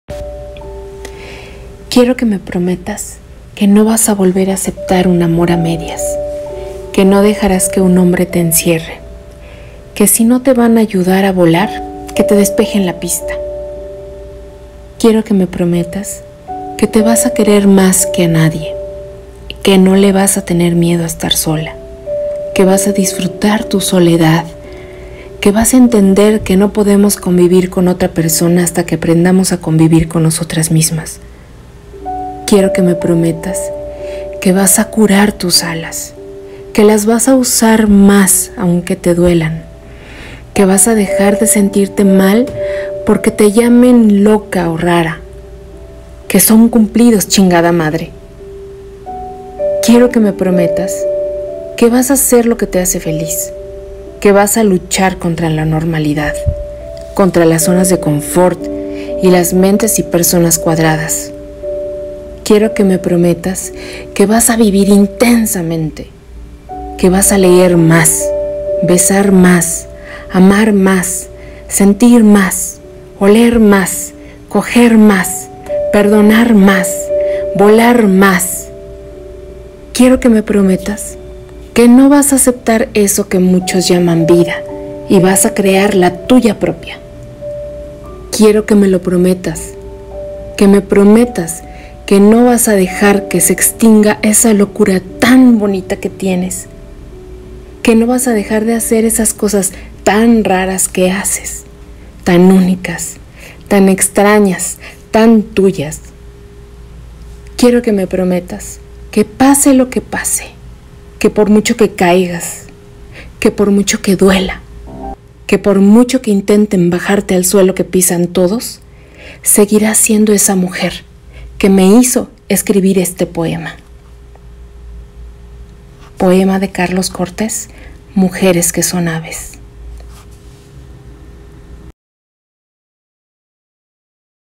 Poema: JURAMENTO… Para ti mujer!!!
Poema maravilloso de Carlos Cortés… dedicado a todas las mujeres… promete que te vas a querer más que a nadie! … 💓